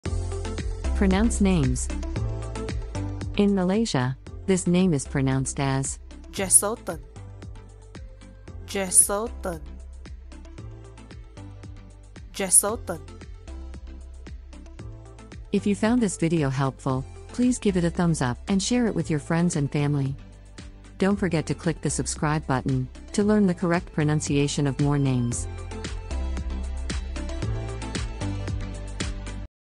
How to pronounce Jesselton in Malaysia? Malaysian pronunciation of Jesselton - Pronounce Names
How do you pronounce Jesselton in Malaysia? Click here to listen to the pronunciation of Jesselton in Malaysian accent.

to learn how to pronounce Jesselton in different countries and in different accents, or for more information about 'Jesselton' such as meaning, origin, etc.